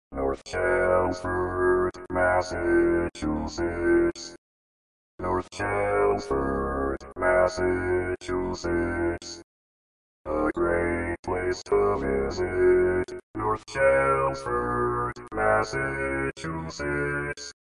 How to Say or Pronounce USA Cities — North Chelmsford, Massachusetts
This video shows you how to say or pronounce North Chelmsford, Massachusetts.

A computer said North Chelmsford, Massachusetts. How would you say North Chelmsford, Massachusetts?